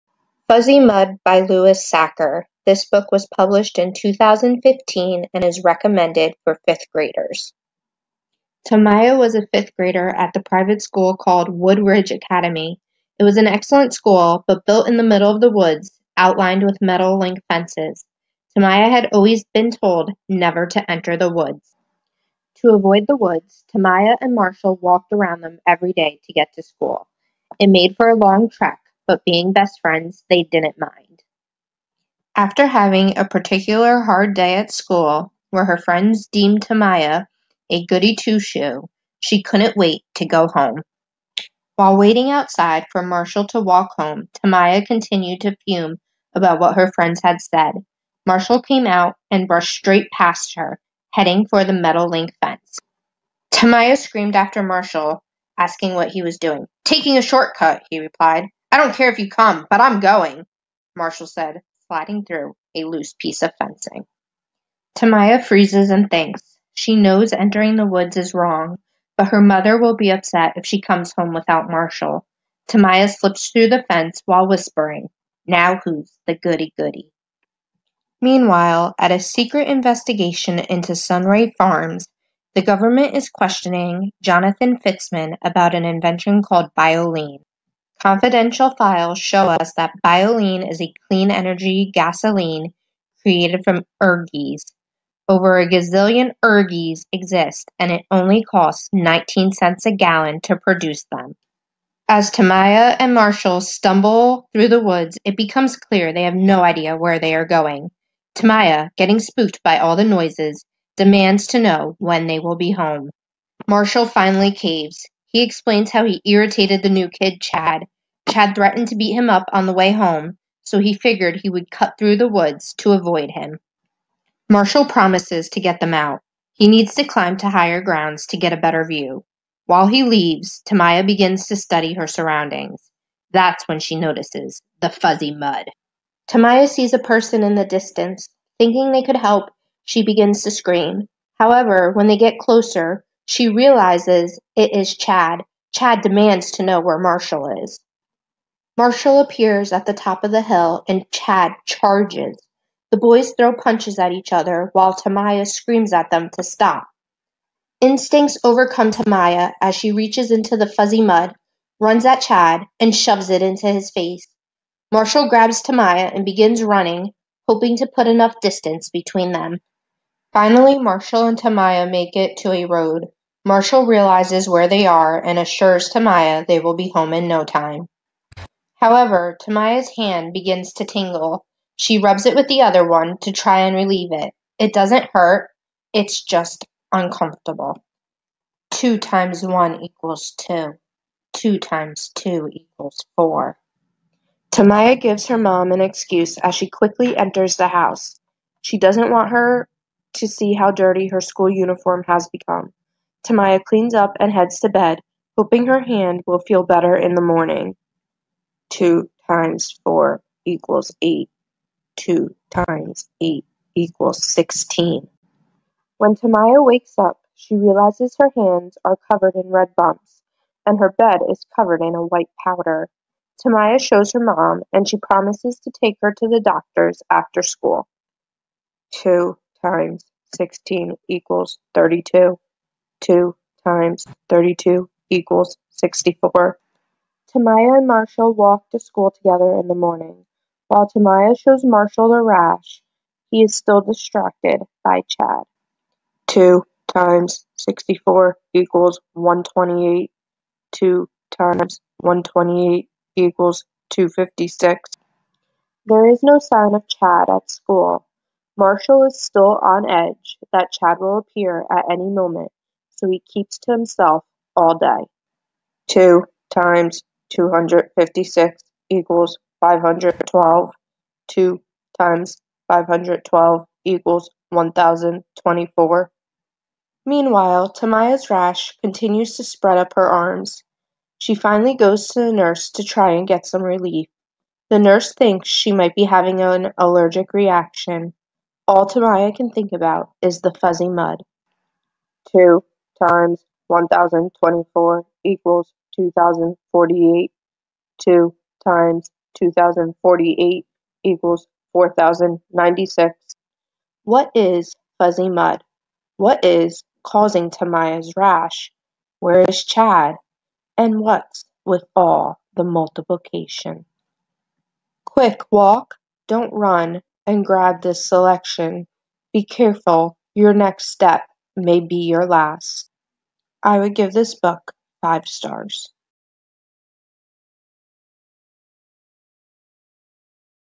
Fuzzy Mud Book Review
This book review was created as the documentary alternative for Rutgers course: Literacy Development: Spring 2018.